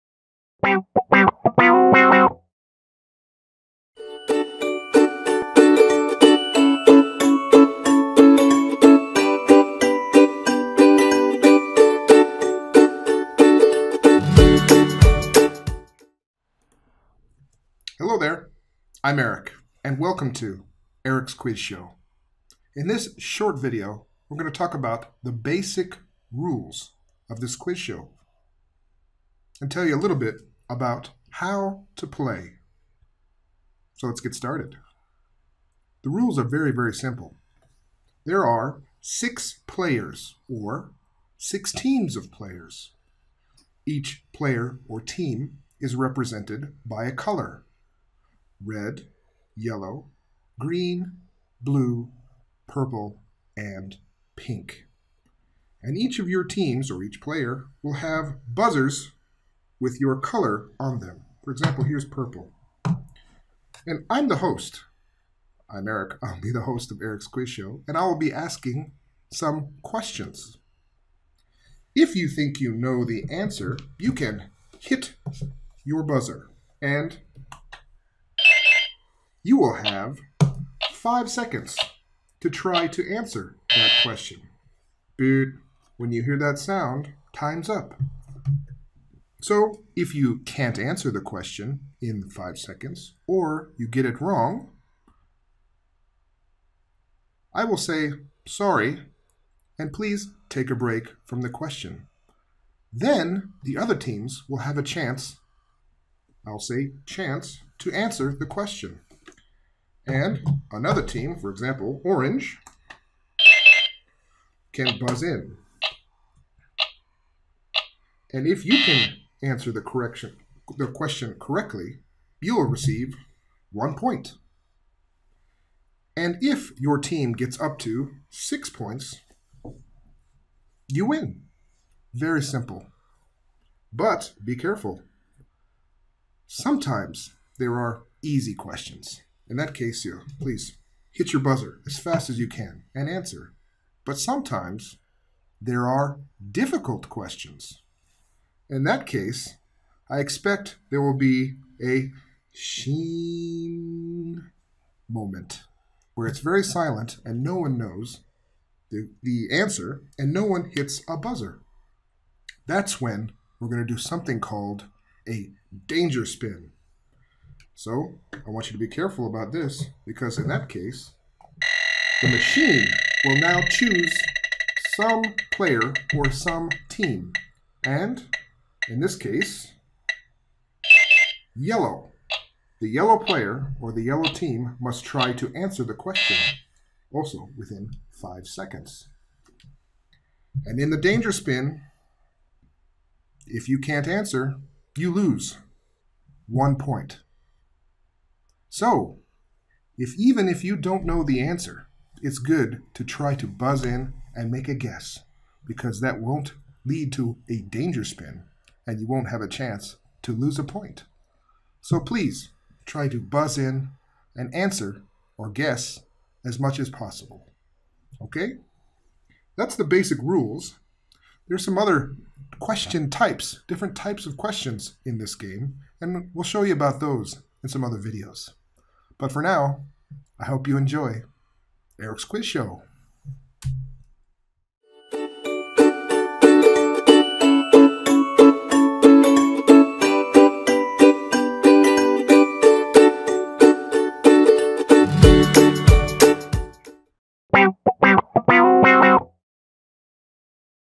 Classroom Quiz Game: Essential Rules
Join @erichawkinson as he walks you through the basics of his quiz show in this engaging and informative video. Perfect for newcomers and seasoned players alike, this video outlines the simple yet exciting rules of the game. With Eric as your host, discover how teams or individual players, each represented by unique colors, compete by buzzing in to answer questions within five seconds. Understand the competitive structure where correct answers score points, and learn about the risk of losing points during the "danger spin" if a question goes unanswered. Whether you’re planning to join the quiz or host your own, this video ensures you're well-prepared to dive into the fun and challenge of Eric's Quiz Show!

Chapters with Timestamps:
Welcome to the Quiz (00:17)
Rules Overview (01:07)
Answering Correctly (02:00)
Risk of Silence (02:55)
Gameplay Strategy (03:44)
Other Question Types (04:10)